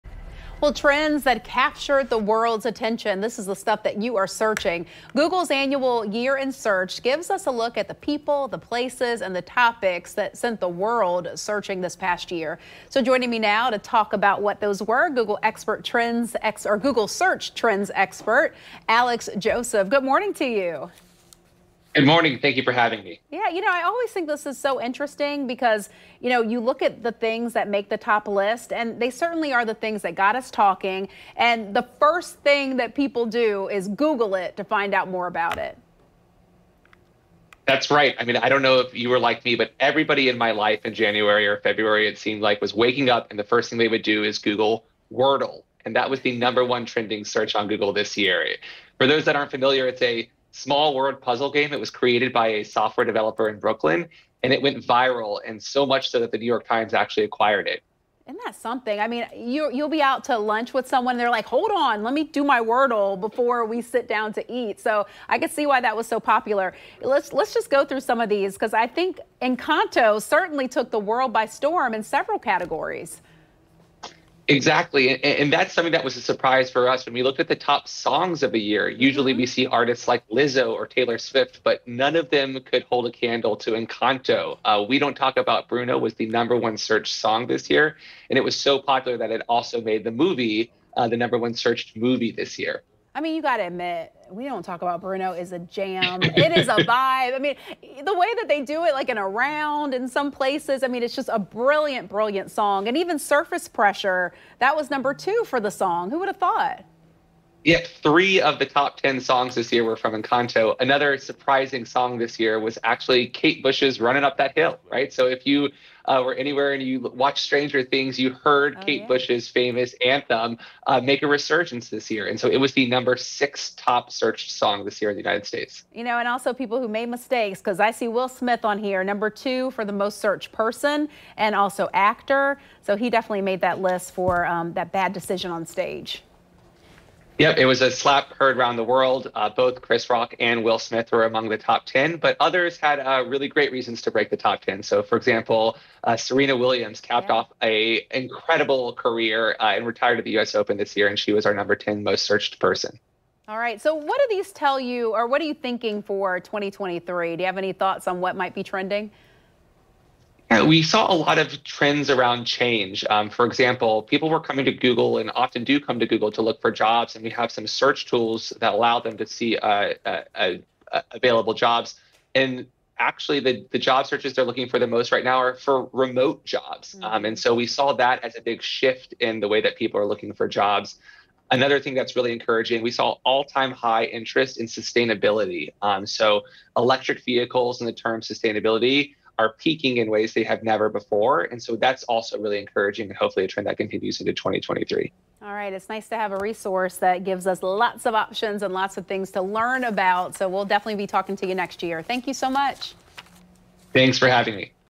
This year's top Google searches
Google Search Trends Expert Alex Joseph joins us to discuss the top people, places and topics that the world has been searching for this past year.